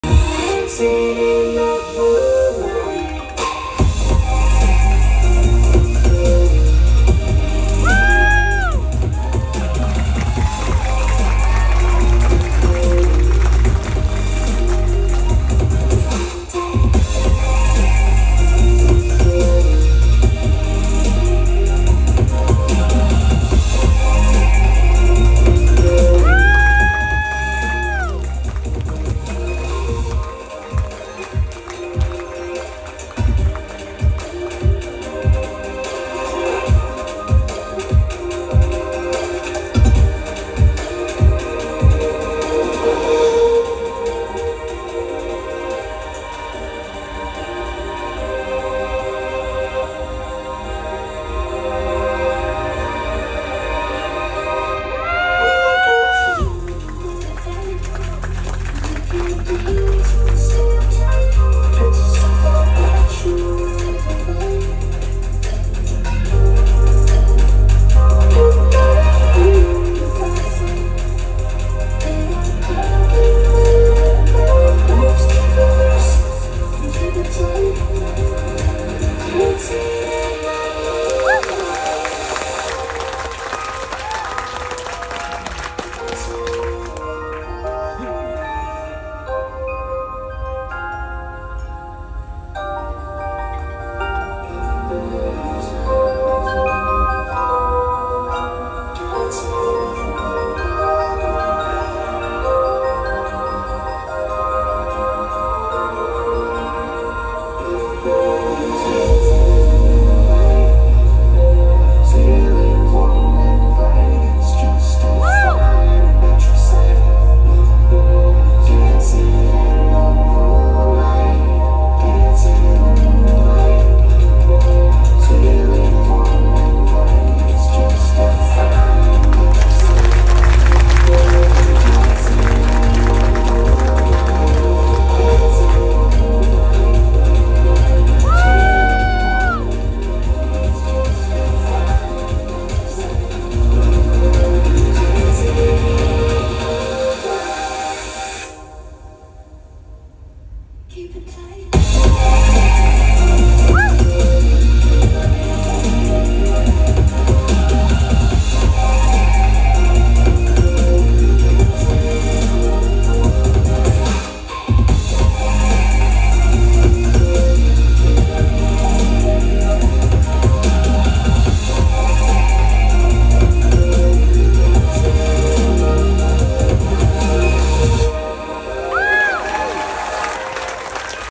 Elke Uhd - Dance Trapeze
Dance trapeze act in Blanc de Blanc Uncorked, onboard the Pacific Encounter Cruise ship. September 2024.